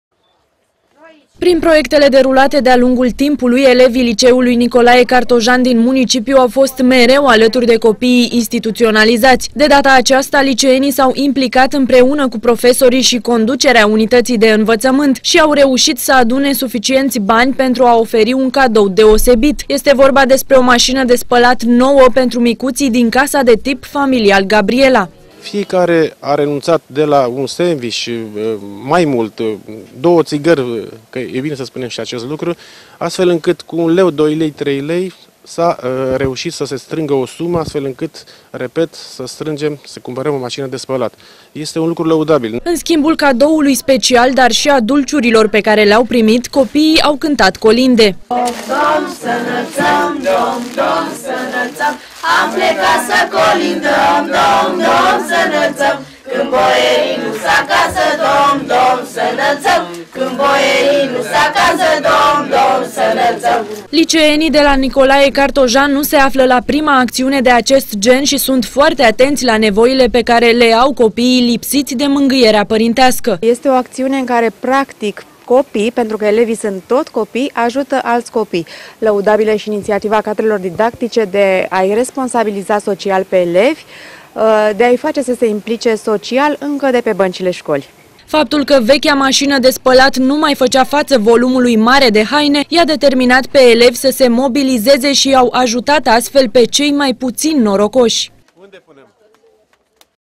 ELEVI IN ROLUL MOSULUI
Moș Crăciun a ajuns astăzi la casa de tip familial "Gabriela" și a adus un cadou special. Pe lângă pungi cu dulciuri, cei 10 micuți cazați aici au primit o mașină de spălat nou-nouță. Ajutoarele Moșului au fost elevii Liceului Nicolae Cartojan care, împreună cu profesorii și conducerea, au reușit să aducă un strop de bucurie copiilor instituționalizați.